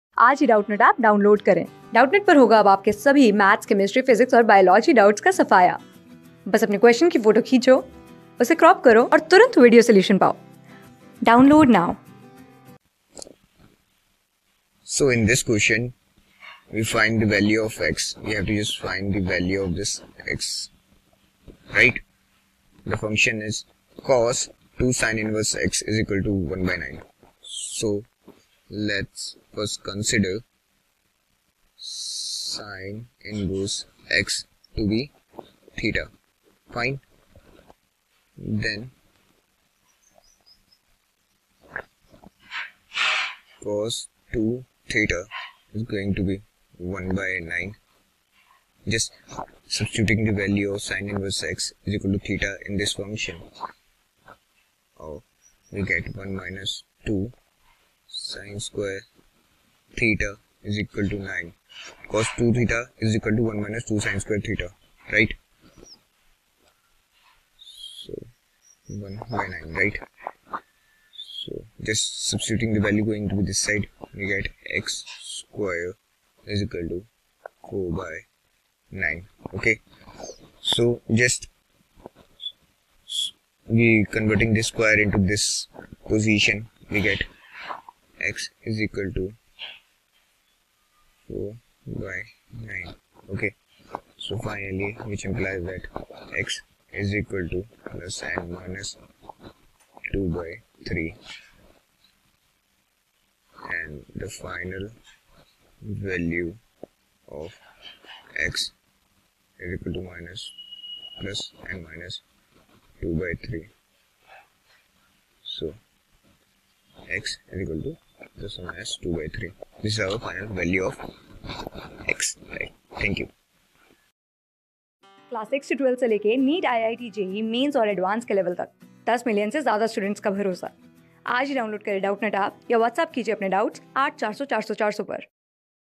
If cos (2 sin^(-1) x) = (1)/(9), then find the value of x | CLASS 12 | INVERSE TRIGONOMETRIC FUN...
If cos (2 sin^(-1) x) = (1)/(9), then find the value of x
Class: 12
Subject: MATHS
Chapter: INVERSE TRIGONOMETRIC FUNCTIONS
Board:IIT JEE

👉 Have Any Query? Ask Us.